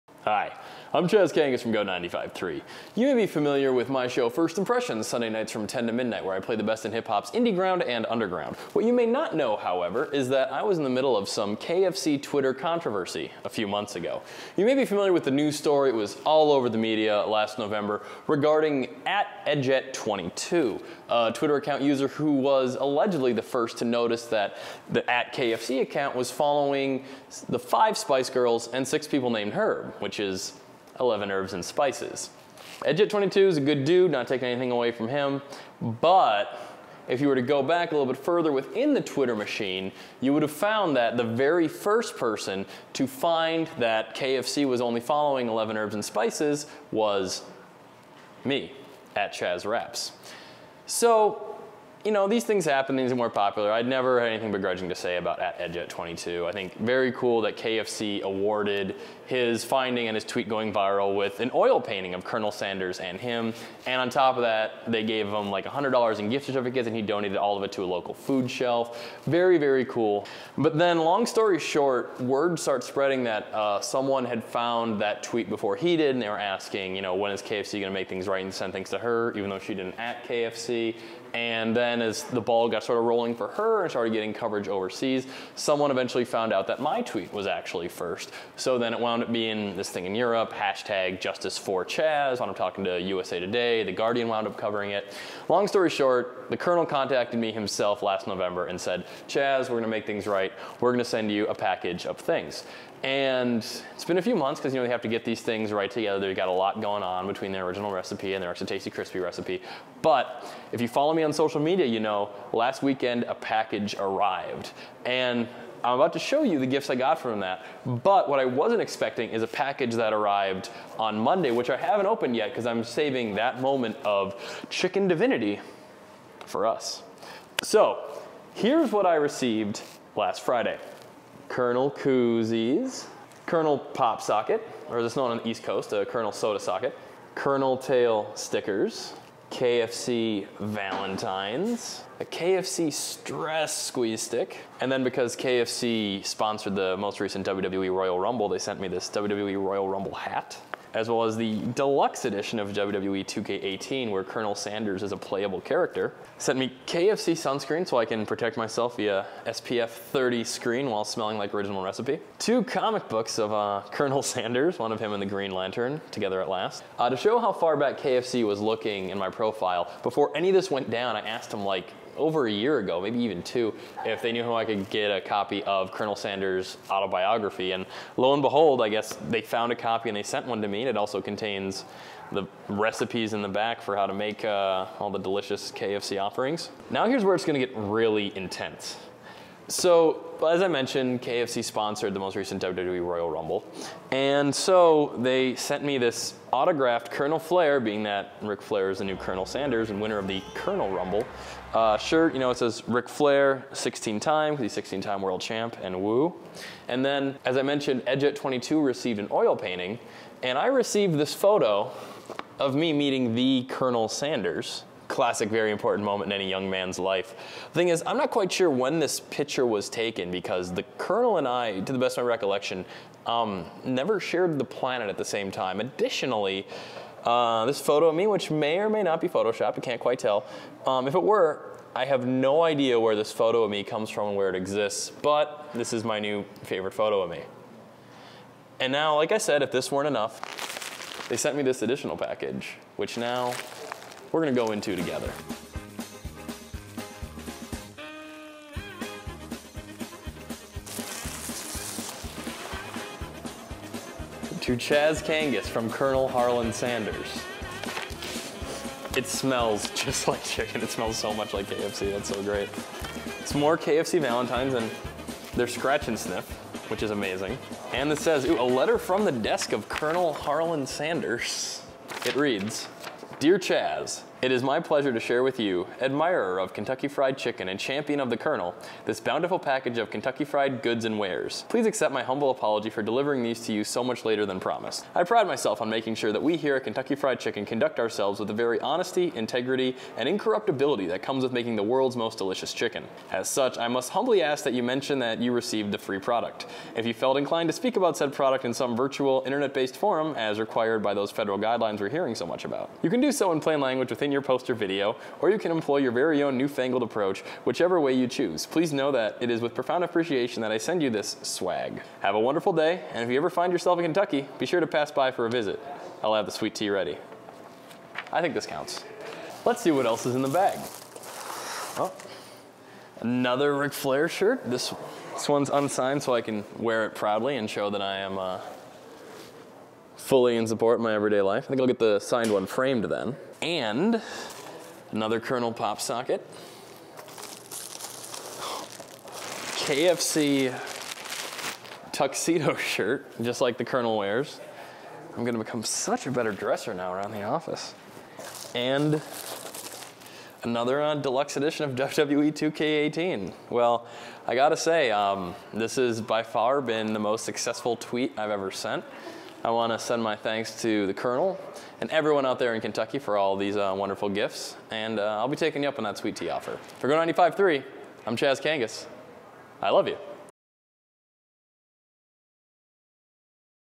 KFC sends Chaz Kangas framed photo and prize package for solving Twitter riddle.
You may have heard last fall about the Twitter user who discovered that the KFC Twitter account was following 11 Herbs and Spices – the five Spice Girls and six guys named Herb. What you may not know is that while that user whose Tweet went viral and resulted in the gift of an oil painting from KFC wasn’t actually the first person on Twitter to catch the clever gag.
Way back in September, the very first was Go 95.3‘s own Chaz Kangas.  As word spread that other users had noticed the followers before the  one that went viral, Chaz was the very first to make the connection.  Being that Colonel Sanders is an honorable man, he promised Chaz he’d make it right.
Six months later KFC came through by sending Chaz two packages  including KFC sunscreen, a KFC stress-drumstick, an autographed KFC/WWE Ric Flair t-shirt and progressively more bizarre items – namely a mysterious framed photo of Chaz meeting Colonel Sanders himself.
Watch Chaz unbox the finger-lickin’ goodies!